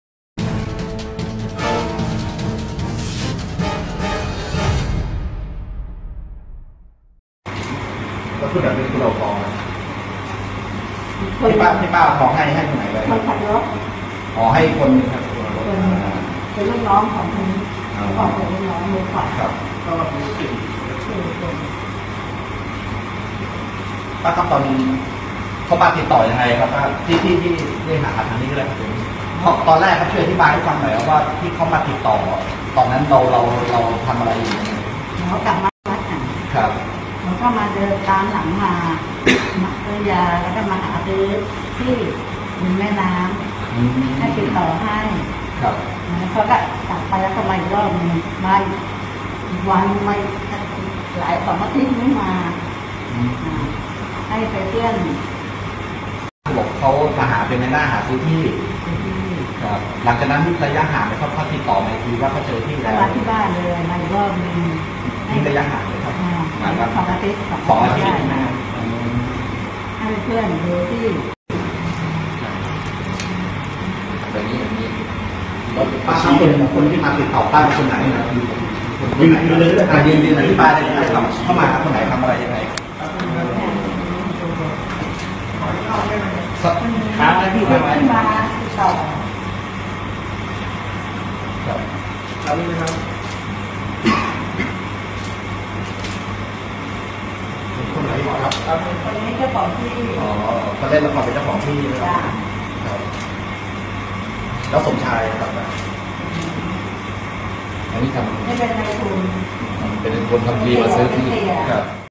รวบสามผัวเมียตั้งแก๊งหลอกซื้อ-ขายที่ดิน เหยื่อหลงเชื่อสูญทอง 12 บาท
17/10/2018 รวบสามผัวเมียตั้งแก๊งหลอกซื้อ-ขายที่ดิน เหยื่อหลงเชื่อสูญทอง 12 บาท
บก.ถปพ.รวบสามผัวเมีย ตั้งแก๊งหลอกเหยื่อซื้อ-ขายที่ดิน ย่านกรุงเทพฯและปริมณฑล สูญสร้อยคอทองคำและสร้อยข้อมือทองคำหนักรวม 12 บาท พบประวัติติดตัว 20 หมาย